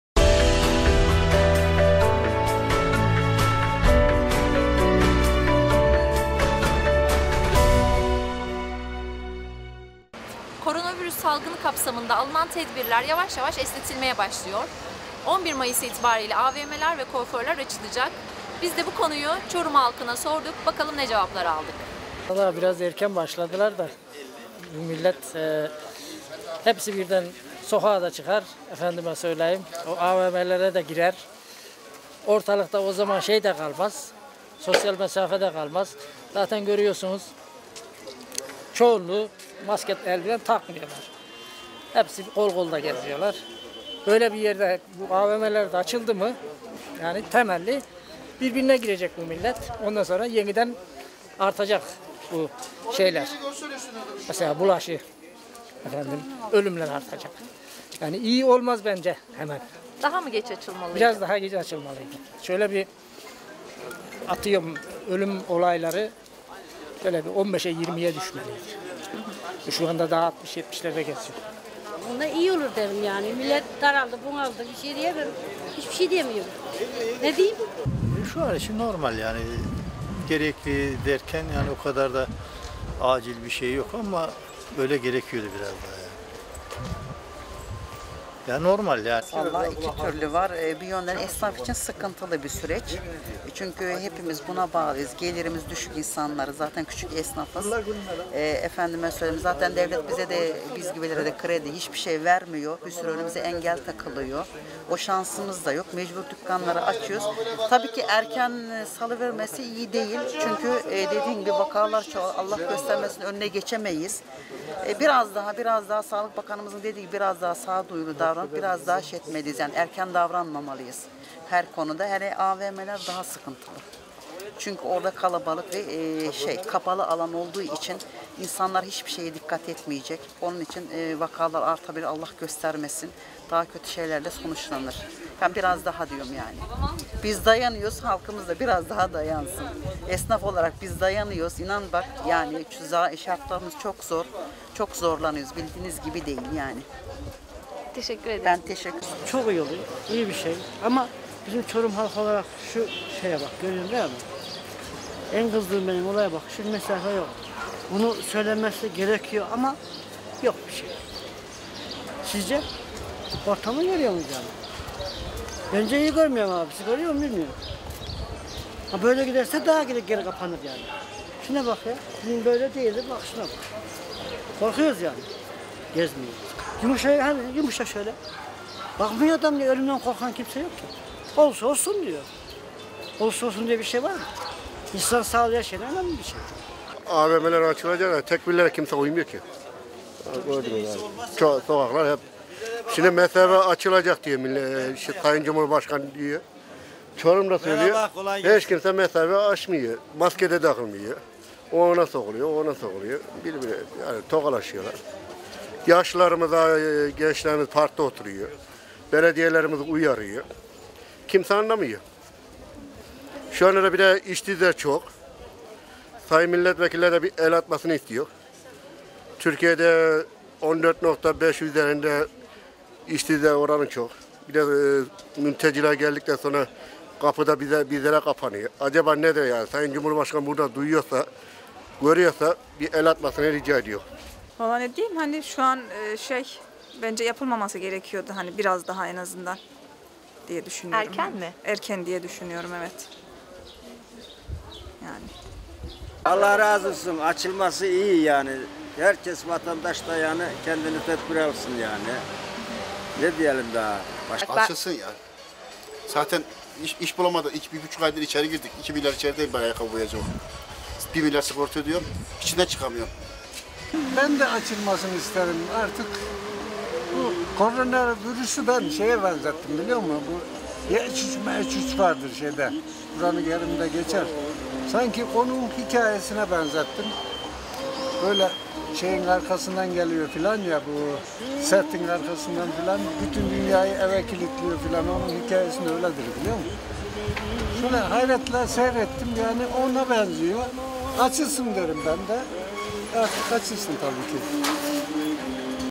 NORMALLEŞME PLANINDA HALK TEDİRGİN
Cumhurbaşkanı Erdoğan, Normalleşme Planı kapsamında atılacak adımları açıkladı. Buna göre 20 yaş altı ve 65 yaş üstüne belirli gün ve saatlerde sokağa çıkma izni verildi. 7 ilde seyahat yasağı kaldırıldı. 25-26 Temmuz’a ertelenen YKS, 27-28 Haziran’a çekilirken, 7 Haziran’da yapılması planlanan LGS ise 20 Haziran’a ertelendi. Alışveriş merkezlerinin, berber ve kuaförlerin 11 Mayıs’ta açılmasına karar verildi.
Peki Çorumlular normalleşme planını nasıl karşıladı. Çorum Haber mikrofonlarına konuşan vatandaşların büyük çoğunluğu, normalleşmeyi tedirginlikle karşılarken bazı vatandaşlar ise “iyi oldu” cevabını verdi.